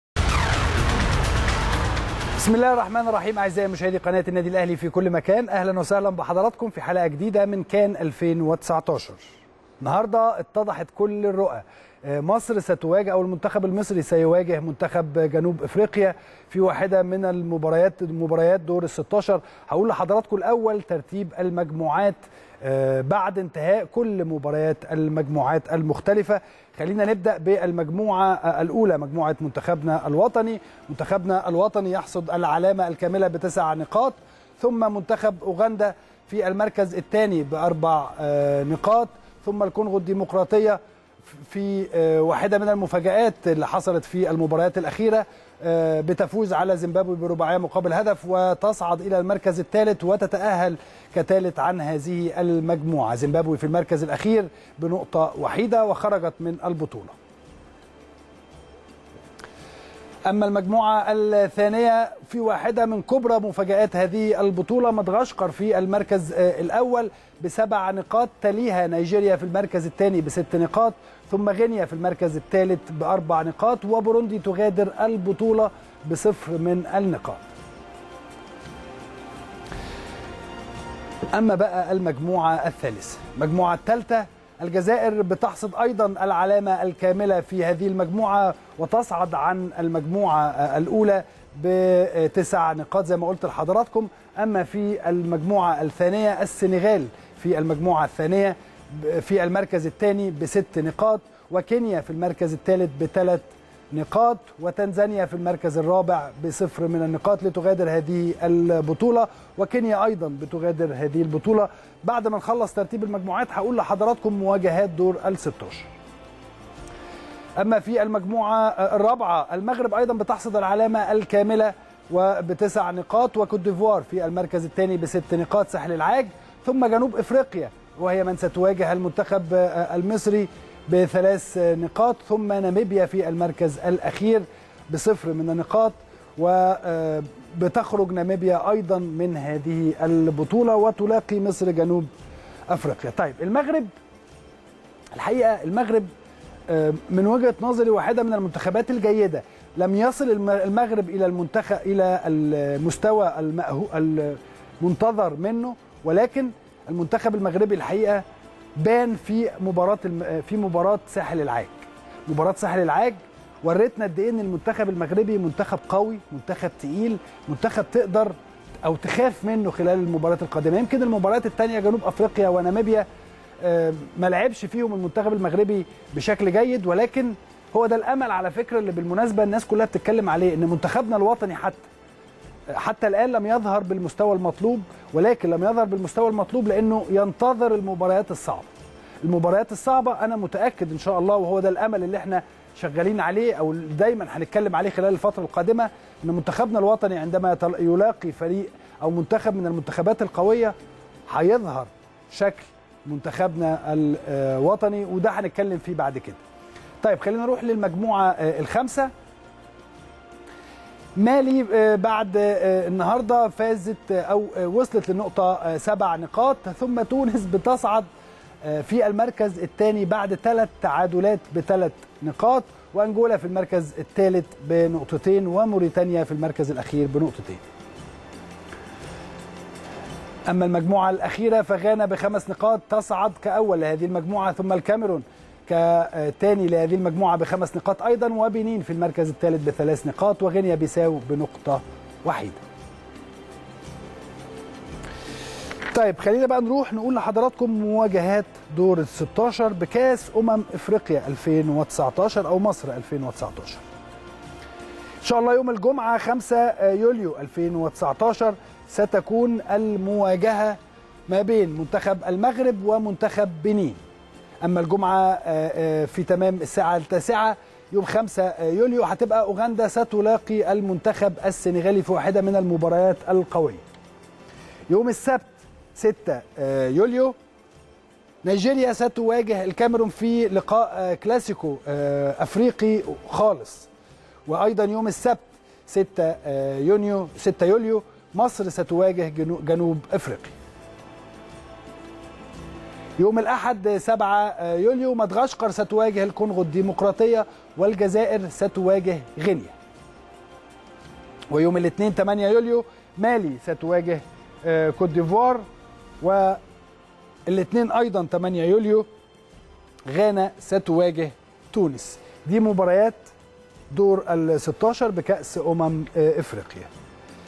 اسلام الشاطر يستعرض مواجهات المنتخبات بدور الـ 16 بأمم افريقيا وجنوب افريقيا تقع امام مصر
التردد: 12341 أفقي.
القمر: نايل سات.
معامل الخطأ: 5/6.
معدل الترميز: 27500